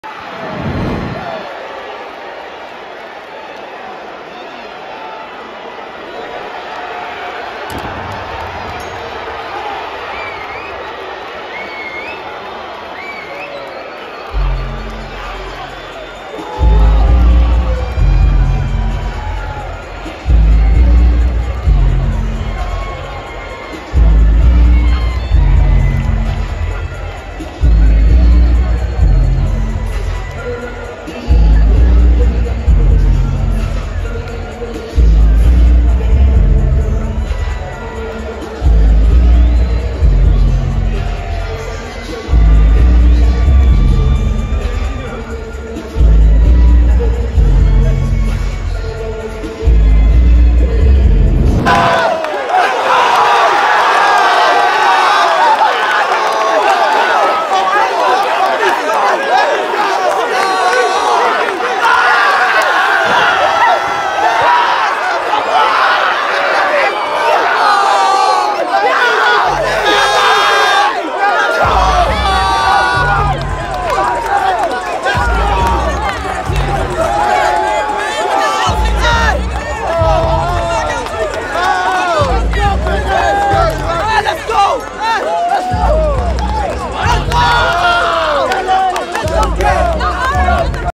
Beta Squad and KSI React To King Kenny KO Win *Multiple POV's*
King Kenny wins against DK Money via first round KO. Beta squad react to his win with KSI.